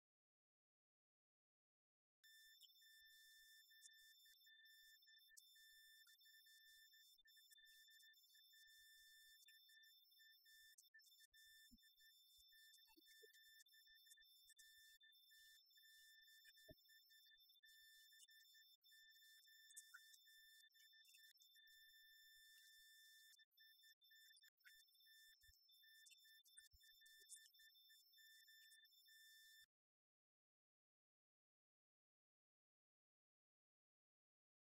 EXO KAYAKS | ZION CREEK | WHITEWATER KAYAKS | P2S2022
Luca from Exo kayaks runs us through the new Exo Zion Creek, their new creeking machine.